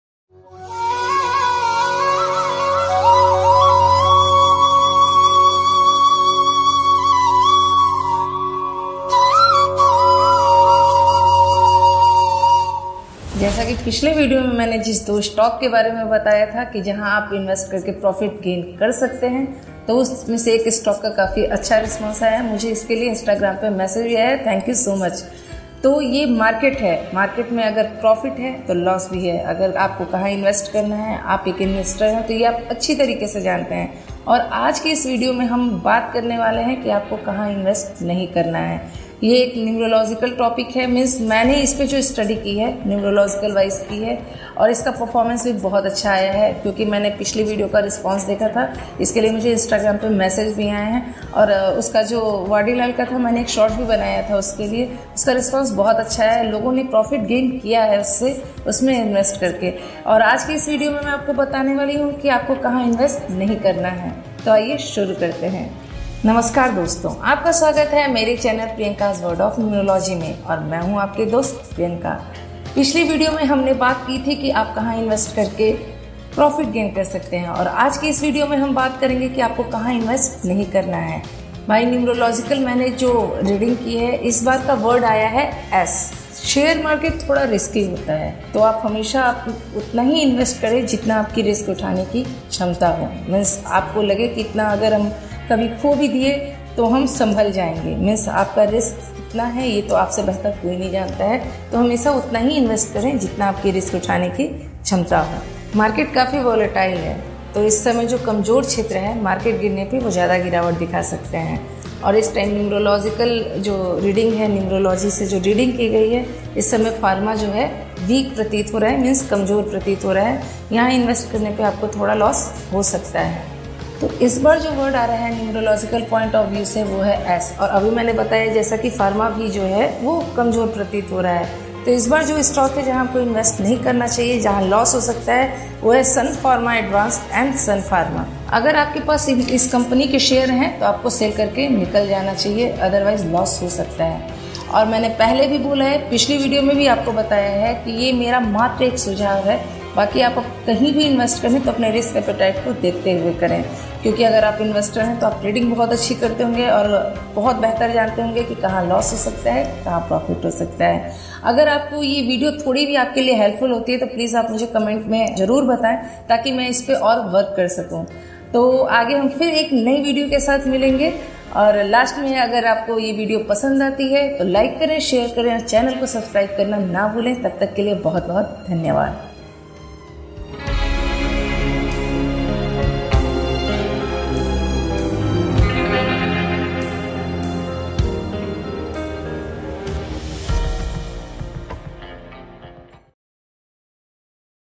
Don't Invest | Investing in Stocks Why I don't | Numerological Point of View
Hello friends in this video I have told in which share market you invest then you can lose.